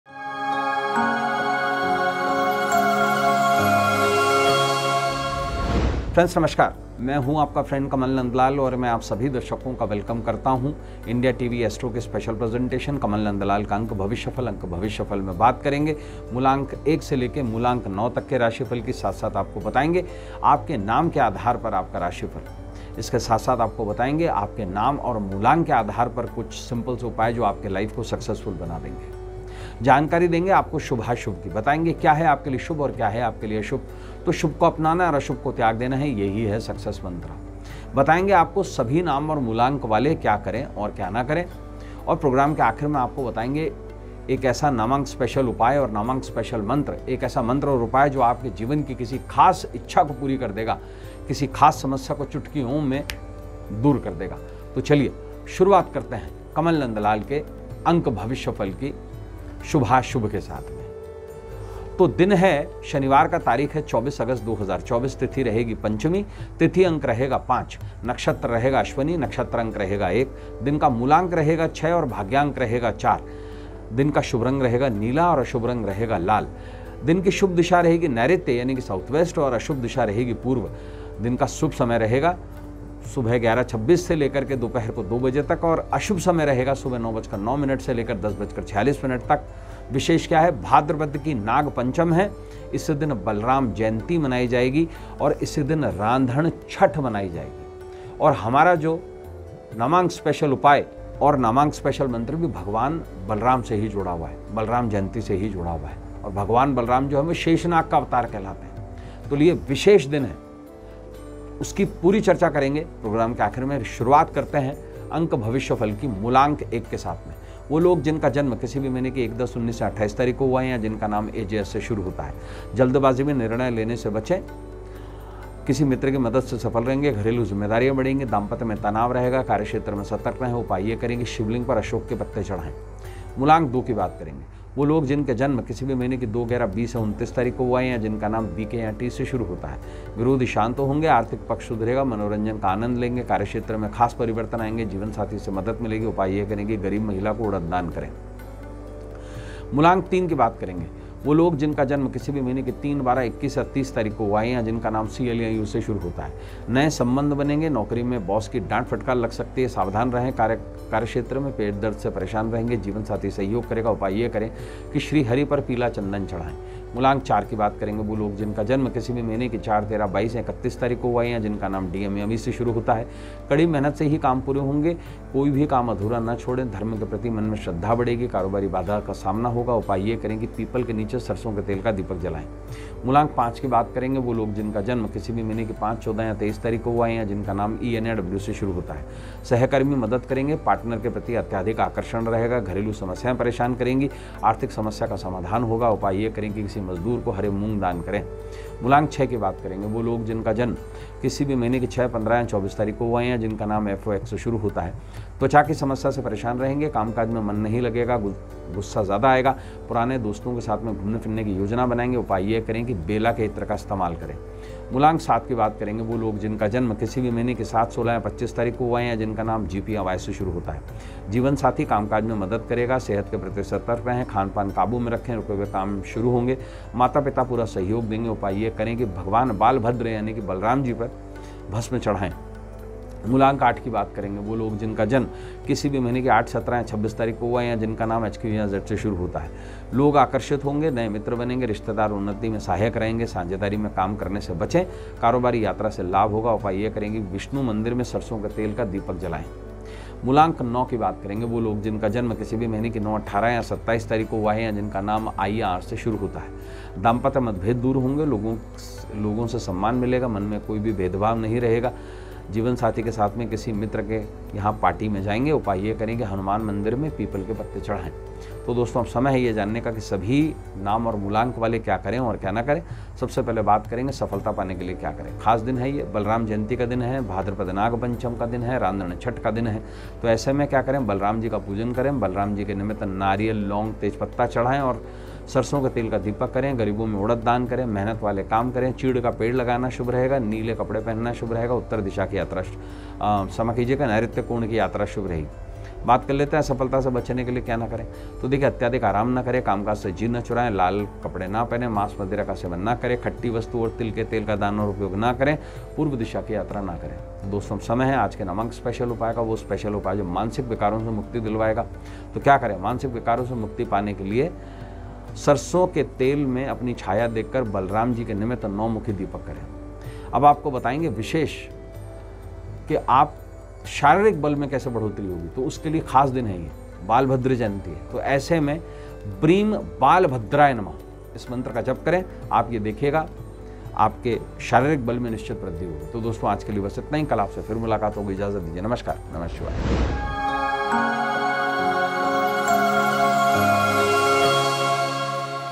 Ank Shastra Aug 24: अंक भविष्यफल मूलांक 1 से लेकर 9 तक का भाग्य, जानिए Kamal Nandlal से | Astrology
अंकज्योतिष किसी के जीवन में संख्याओं का अध्ययन है। किसी व्यक्ति के जीवन काल में अंक बहुत महत्वपूर्ण भूमिका निभाते हैं। यह न केवल किसी व्यक्ति के मार्ग को आकार देता है बल्कि व्यक्ति को उसके जीवन और दिन में अभूतपूर्व कठिनाइयों से सुरक्षित रहने की भविष्यवाणी और चेतावनी भी देता है। अंकज्योतिष पढ़ने में बहुत सारी गहन गणनाएँ शामिल होती हैं।